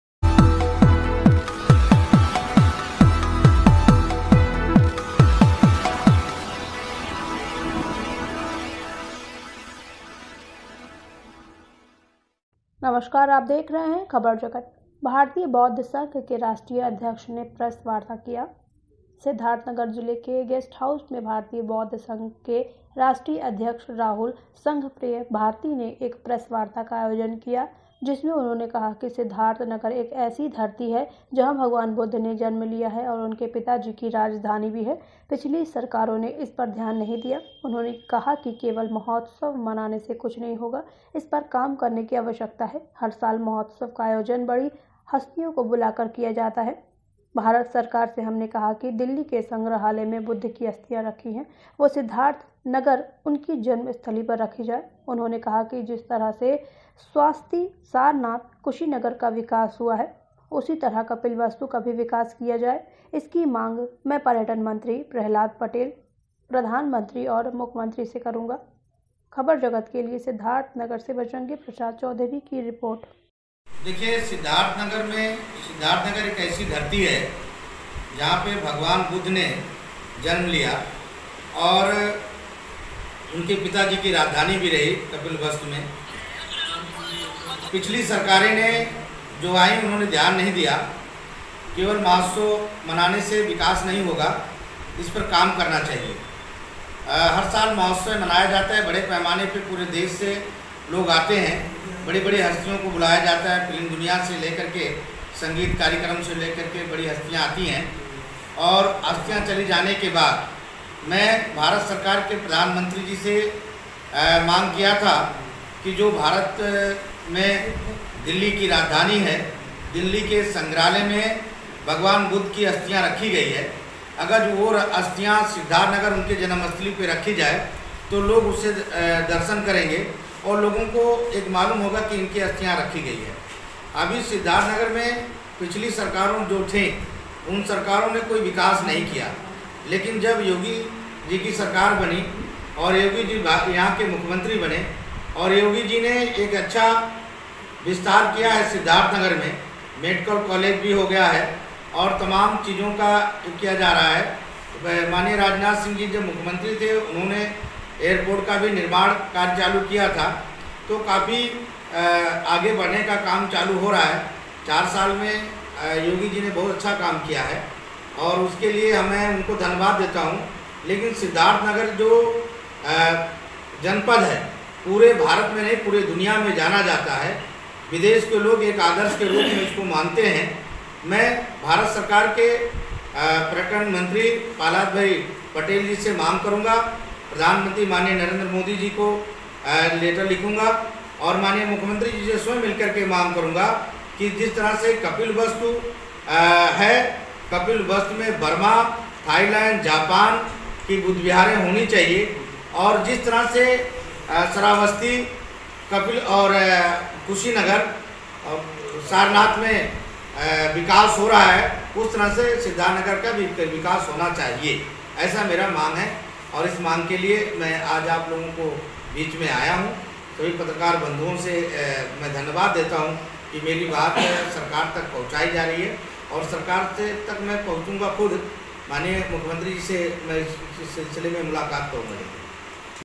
भारतीय बौद्ध संघ के राष्ट्रीय अध्यक्ष नेप्रेस वार्ता किया
भारतीय बौद्ध संघ के राष्ट्रीय अध्यक्ष ने
प्रेस वार्ता  किया